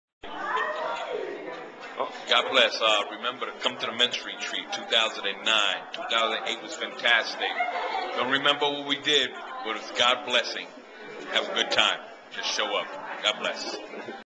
Crossroads 2006 Men's Retreat Foot Prints Mural PT 8
Brother Marty humorously shares...he loves the men's retreat...just couldn't remember what he wrote...lol...come on over anyways...